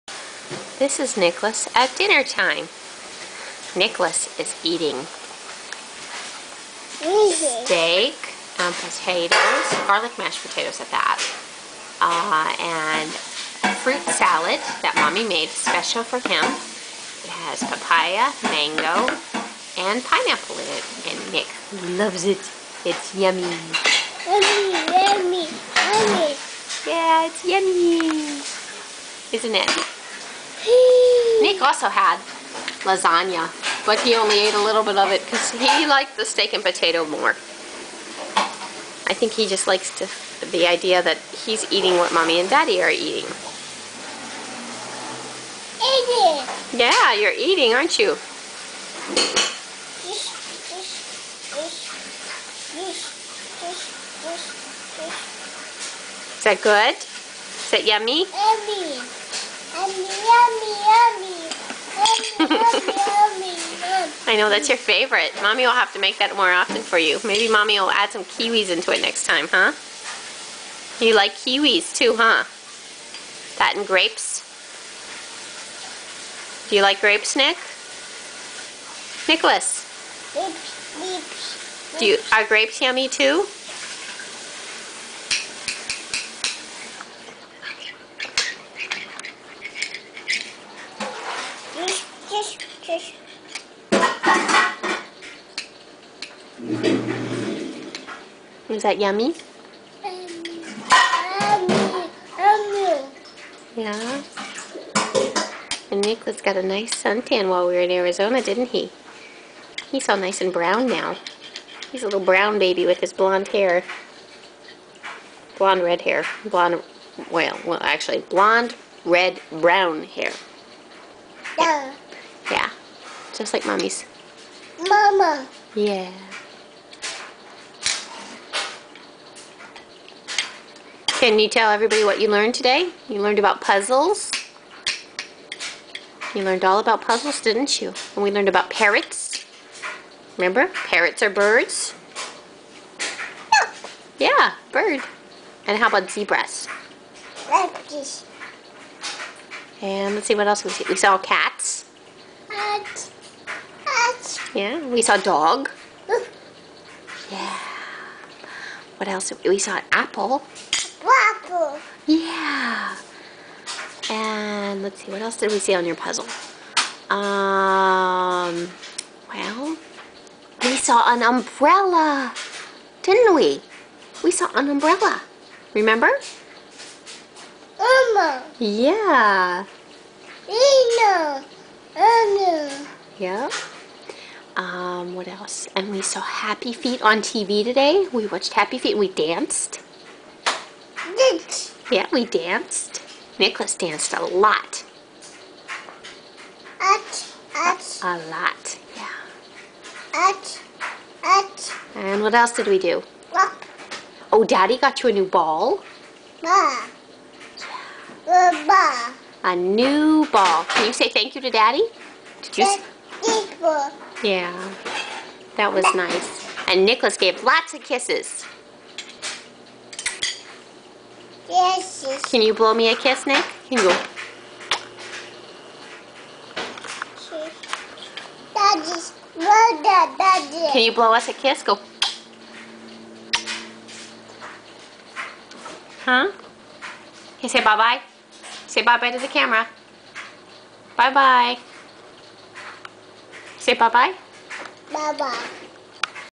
What did we learn today?
After dinner we talked about all the things we did that day.  Puzzles, apples, umbrellas, and such.  Plus a visual tour of Nicholas's dinner.  Oooohh fun!